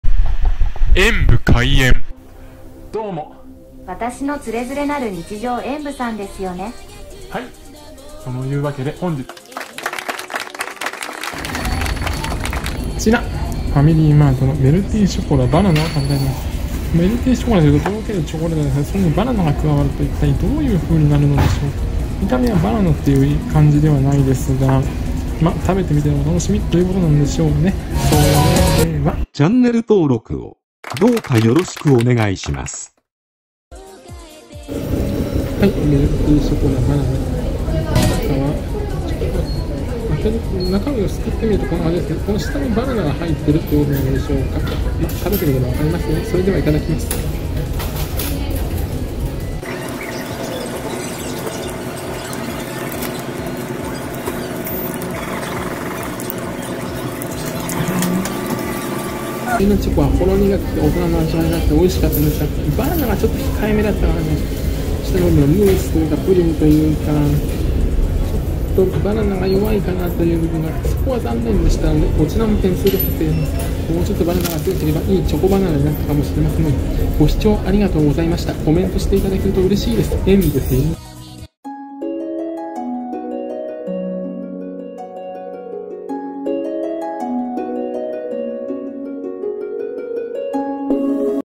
ファミリーマート バナナメルティショコラを食べてみたらバナナ感は……？
ファミリーマート　バナナメルティショコラを食べてみました。見た感じはバナナ感無い様な気もしますけど食べてみると～